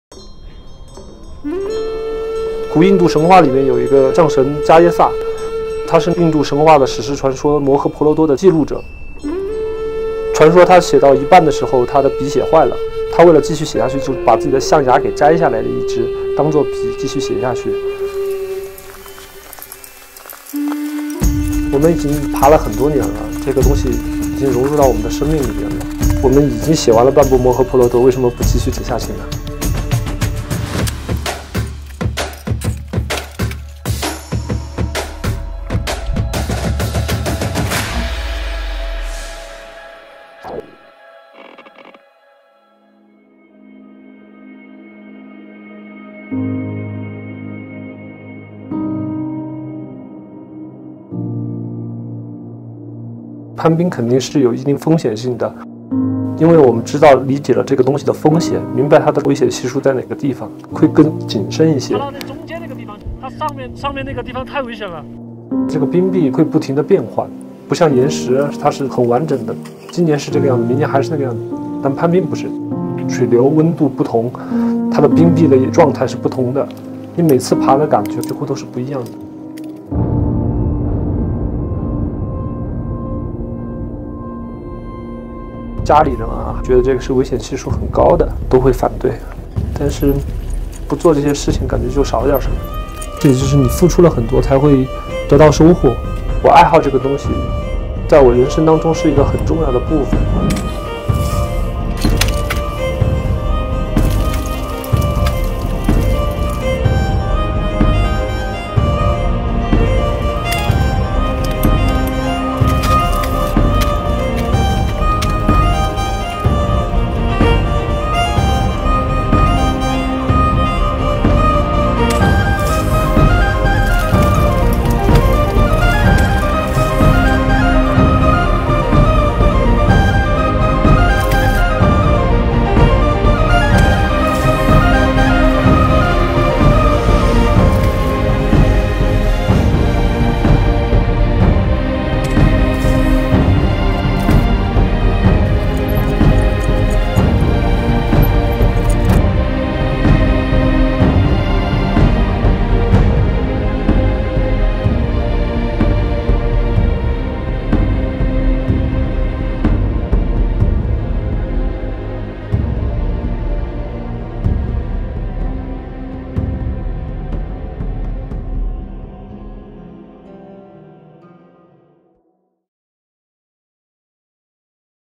【敢玩iDareX】（Climbing ice）攀冰：我攀爬的不只是冰，更是我人生的荣誉
同一地点的不同时段，不同地点的不同情况都让攀冰的物理条件充满了不确定性，这种不确定就像山头摇曳着的蓝色火焰，诱惑着攀冰运动者向上爬去。身后是充满未知的危险，身前是触手可及的荣誉，攀冰者们从不退缩。
喜欢我的视频，多多订阅分享评论哦~